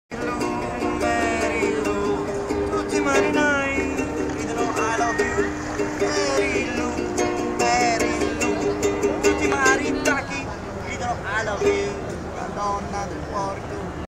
Bangkok Train Station
Bangkok Train Station. FJ Soulard, italian folk singer, offers a sublime performance to a big audience